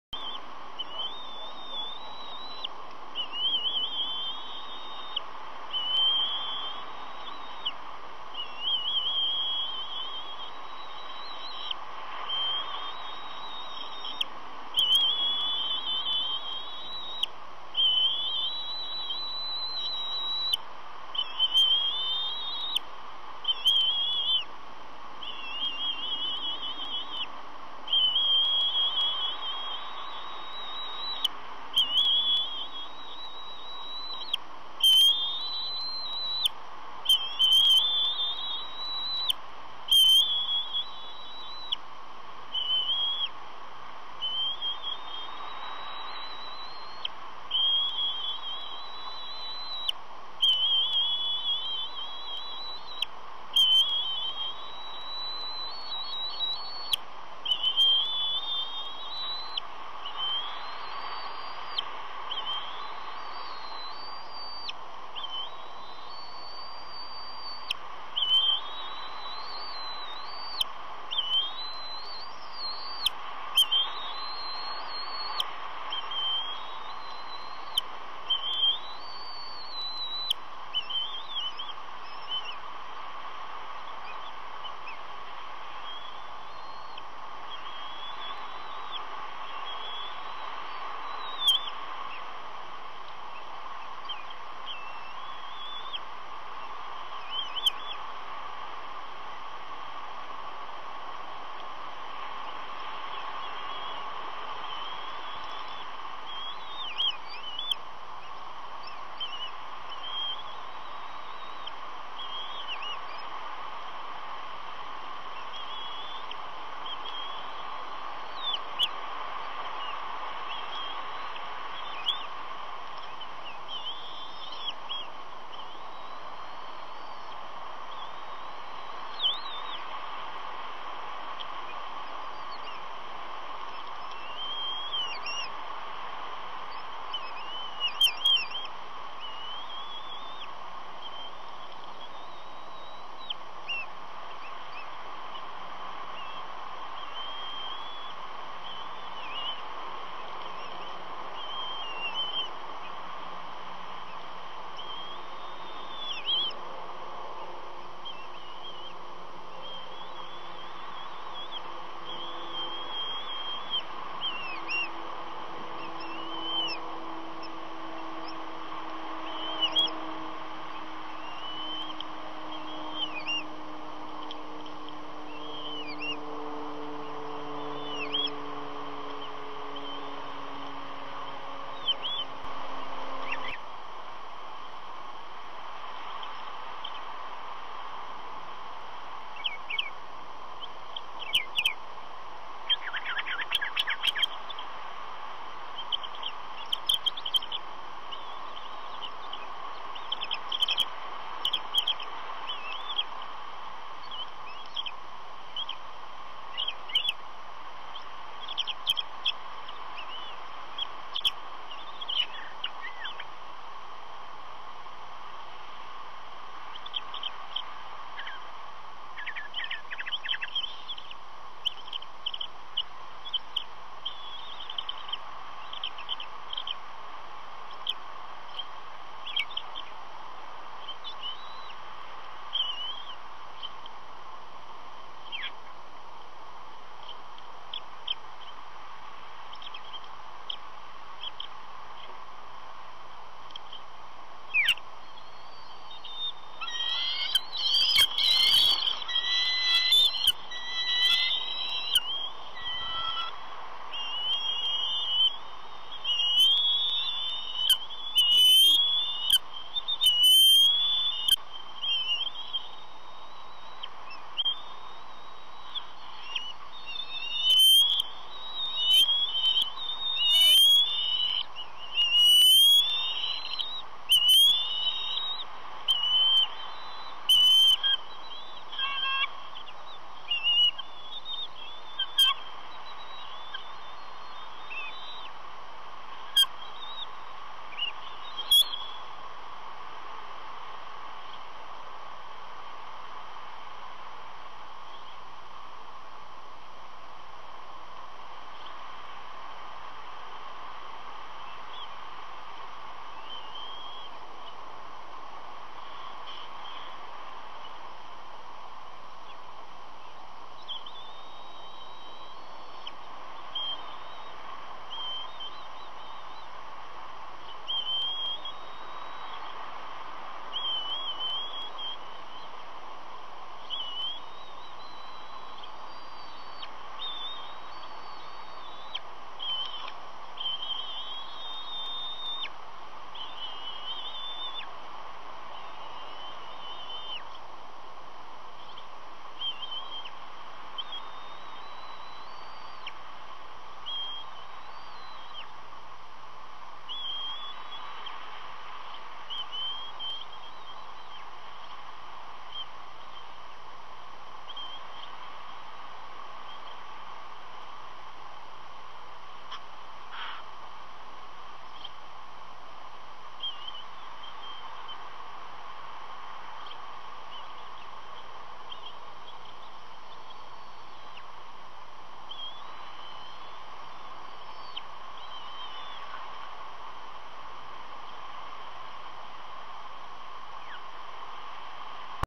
TH: K-81 does not want to share with K-82!
K-81 has delivered a large fish which he is feeding to Jr. But when K-82 arrives he doesn't want to share with her .. so he takes his fish and leaves!  To find out more about the Restoration Project on Channel Islands off California go to iws.org